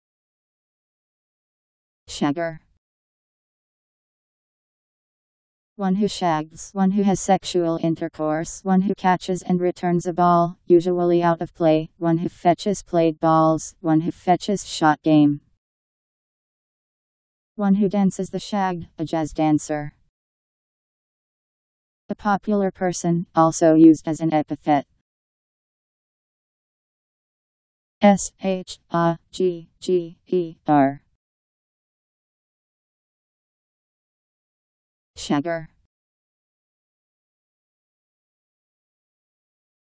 Shagger Meaning
Video shows what shagger means. One who shags.
One who has sexual intercourse.
One who catches and returns a ball, usually out of play, one who fetches played balls, one who fetches shot game.. One who dances the shag, a jazz dancer.. A popular person, also used as an epithet..  Shagger Meaning. How to pronounce, definition audio dictionary. How to say shagger. Powered by MaryTTS, Wiktionary